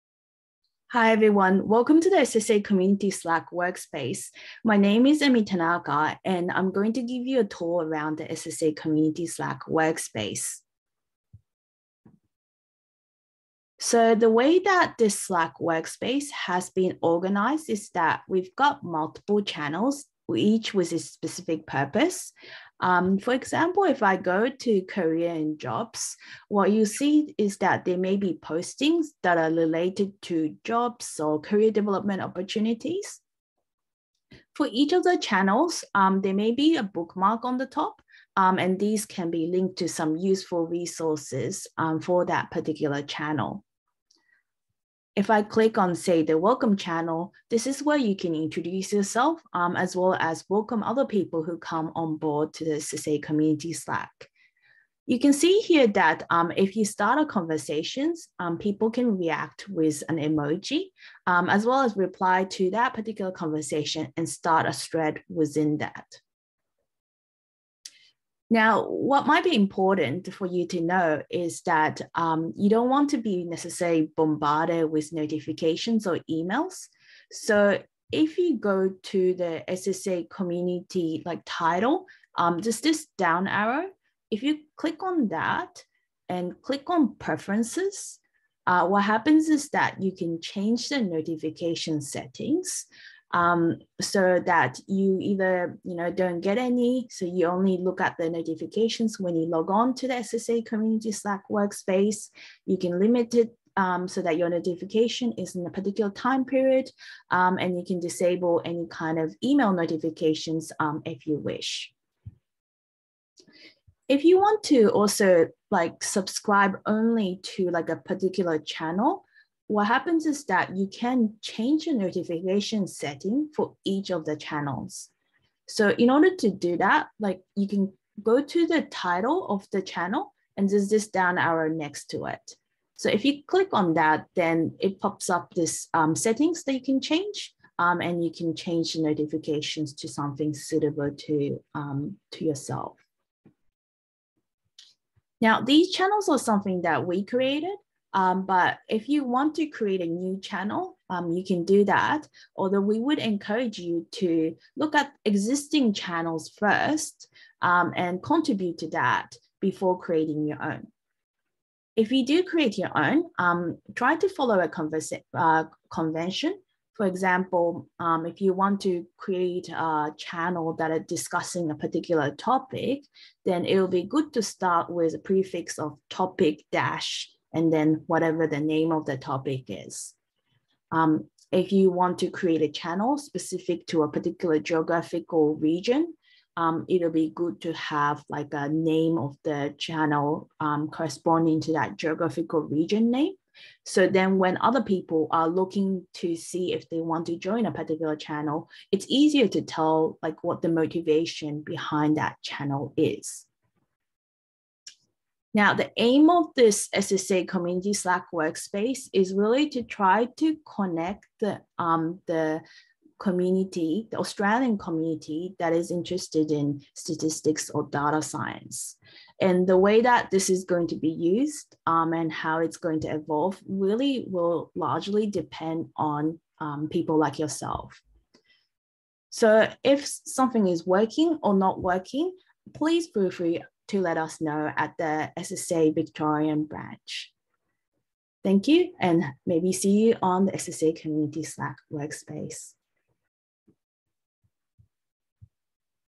Introducing the SSA Community Slack
The aim of the SSA Community Slack is to connect the Australian community interested in statistics and data science. This video gives you a short tour of the Slack workspace. If you would like to join or see more information, you can find this